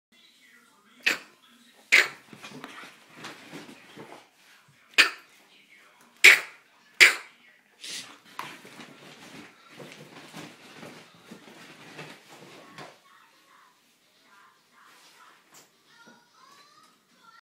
Whippet goes crazy !
Un whippet nommé Di Nozzo en pleine folie, nous présente sa technique dite du Tourniquet ! Je paris que vous n'arrivez pas à en faire autant sur votre lit :D